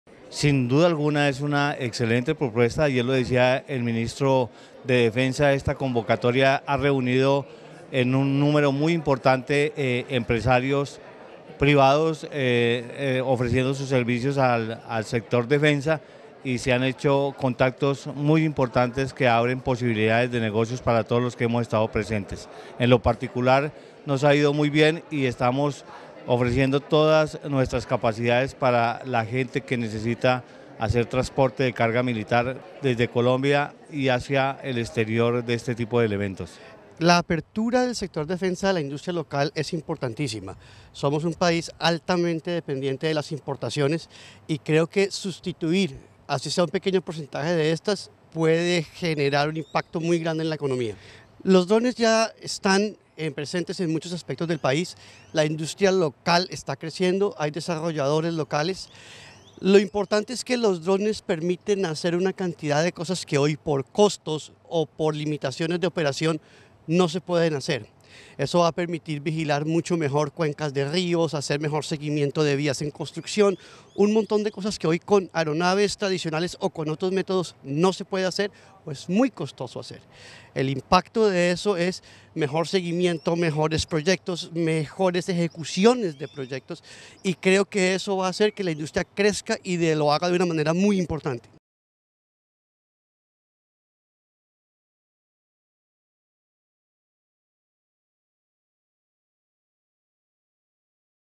En Cartagena se dieron cita industriales privados y el Sector Defensa
Nuevas oportunidades de negocio y mejor conocimiento del sector de la industria del sector defensa, fueron algunos de los logros del Encuentro de la Industria Privada y el Sector Defensa que tuvo lugar en el Club Naval de Cartagena.